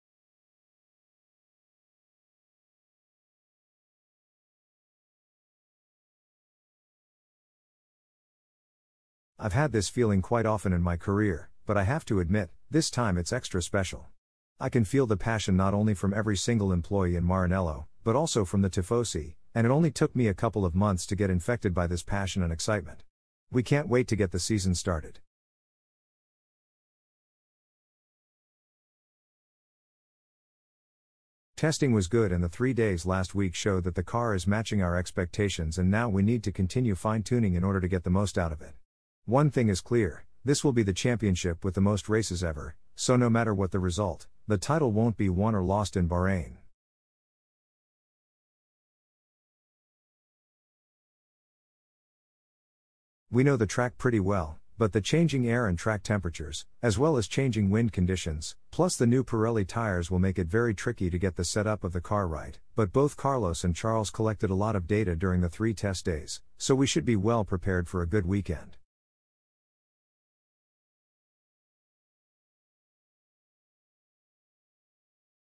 Three questions to... Frédéric Vasseur
And they’re off! The 74th Formula 1 World Championship season gets underway in Bahrain, the longest ever with 23 races on four continents. The 5.412 kilometre-long Sakhir track is a very familiar one for the drivers as it was used twice in the same season in 2020, while it has also seen hundreds of laps completed during pre-season testing.